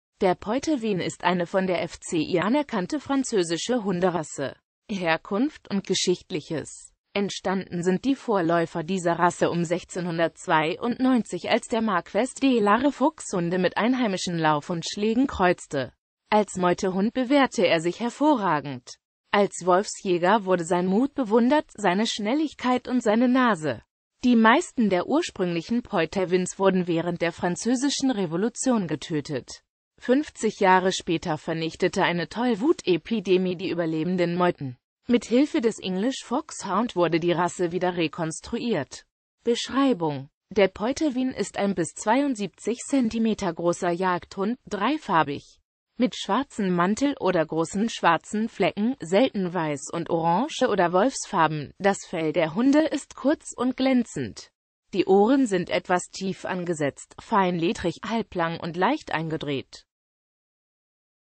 Poitevin (Hunderasse)
Vielen Dank für Ihre Unterstützung:

Der Poitevin ist eine von der FCI anerkannte französische Hunderasse .